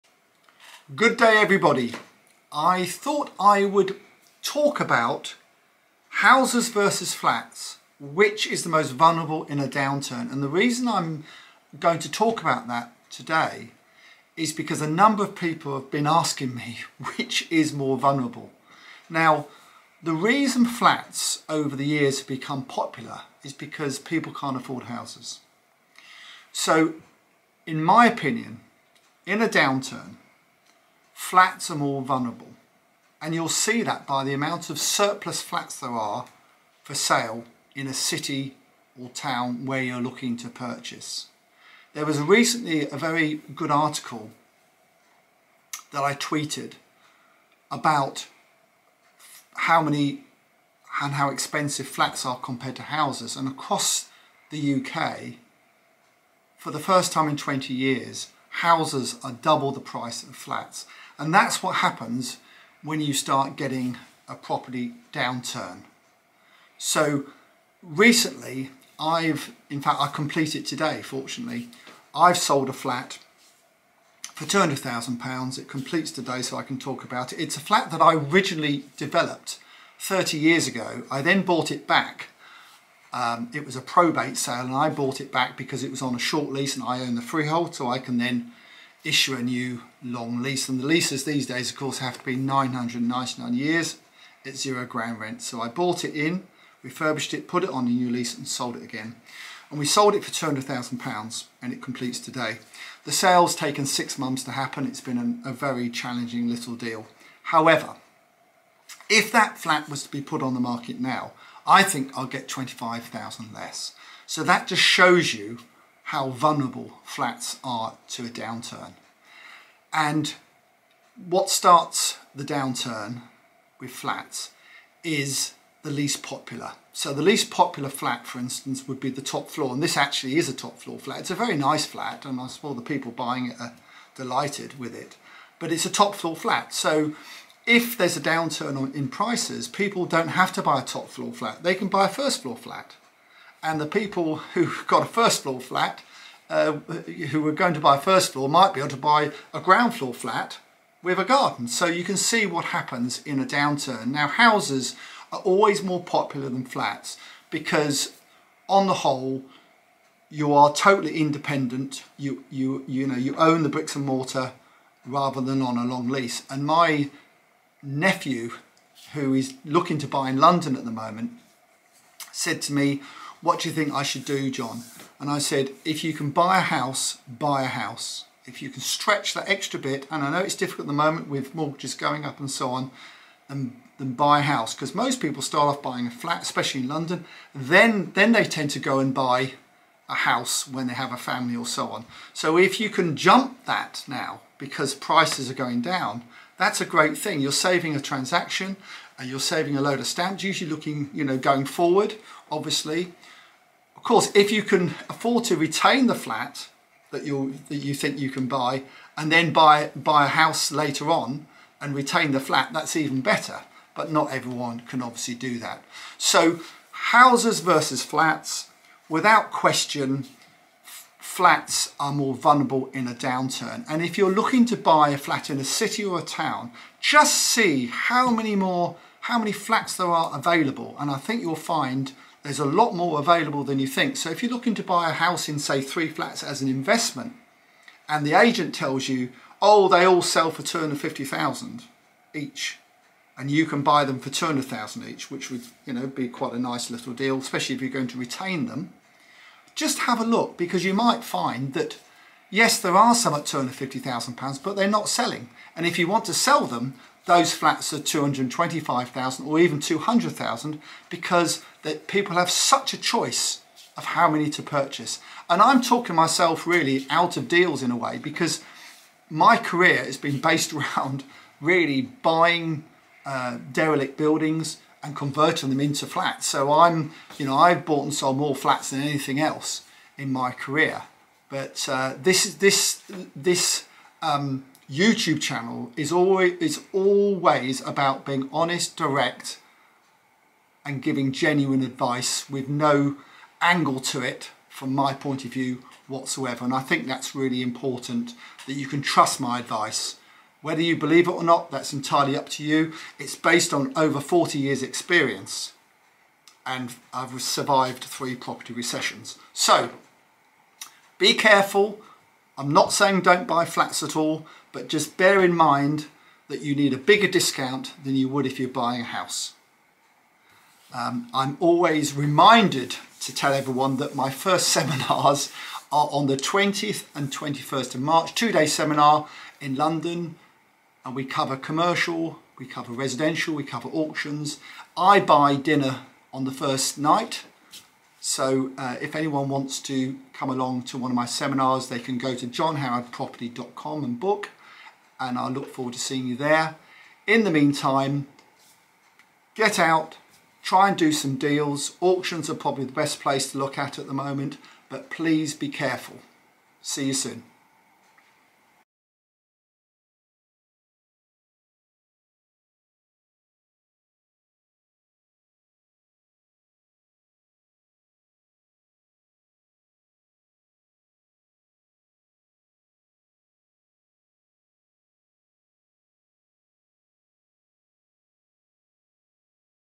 The Best Property Investment For 2022? Flats vs House | John Howard Property
Today I am speaking about house vs flat prices, the market forces that affect both and what you should be investing in during a downturn. Watch the video to find which is more vulnerable?